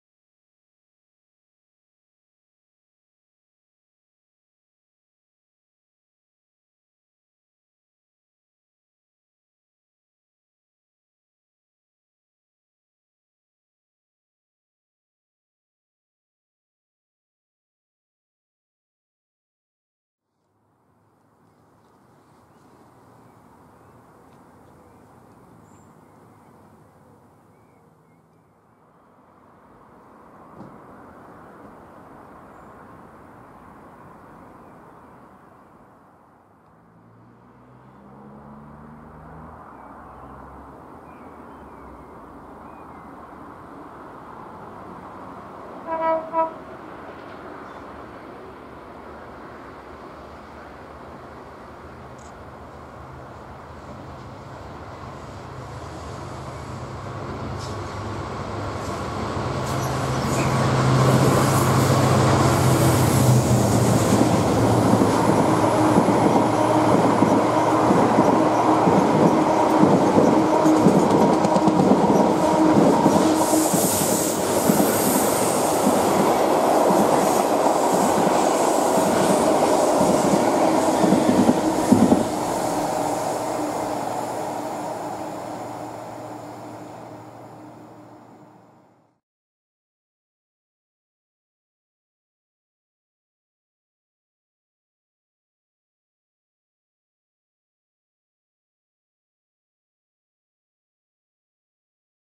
First Class 701 through Totton with x2 Class 66s - 14/06/2020
The first movement of an SWR Class 701 EMU down the Wessex Line from Eastleigh to Wareham was seen on the evening on Sunday 14th June 2020 when GBRf Class 66 'Shed' Co-Co Nos.66775 "HMS Argyll" and 66745 "Modern Railways" top-n-tailed a special train including the new unit down the line.

In this short video we see the convoy passing through Totton.